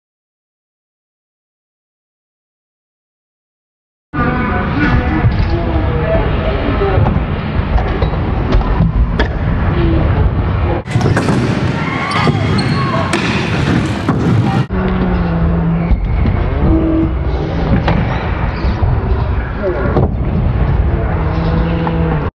3 xc clips with Dan
a few clips of a kid who i ride with at xc.every thing was first try he tried a briflip air but we thought to not add it in.dan has also improved a lot in a short time.